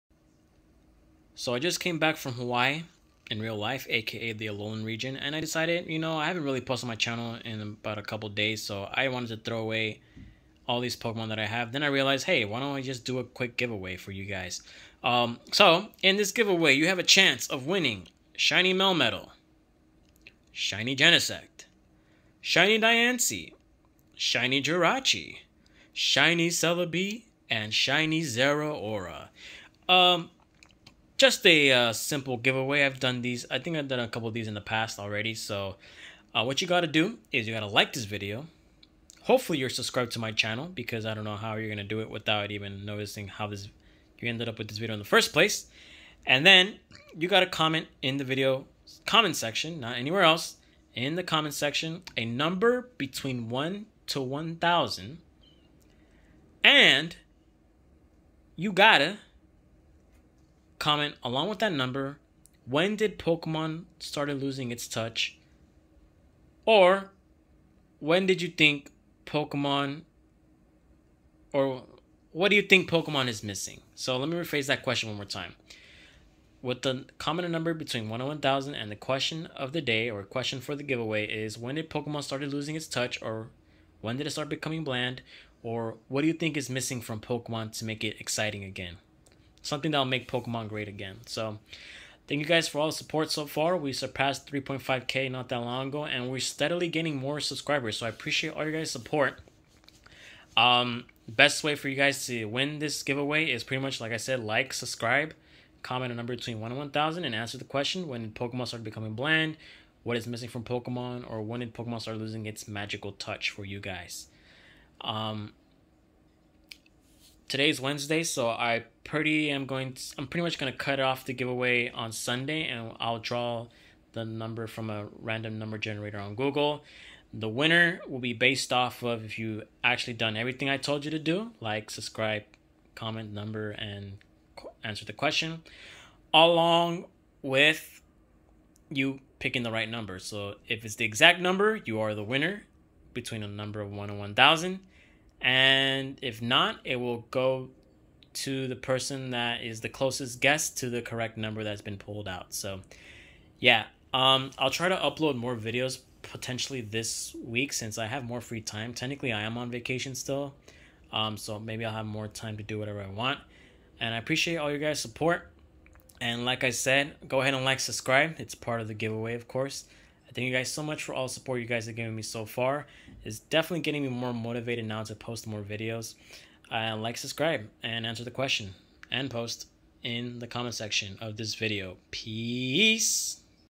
Crazy Shiny Event Box Giveaway! (Read Description) [CLOSED]
Giving away a box of Shiny Event Pokemon that was leftover from the previous giveaway. Rules are simple, so read them:

1. Like this video!
3. Comment a number between 1 - 1000
4. Along with your number, answer this question in the comment section: when did Pokemon lose its touch/when did Pokemon started becoming bland/when did pokemon lose its magic touch?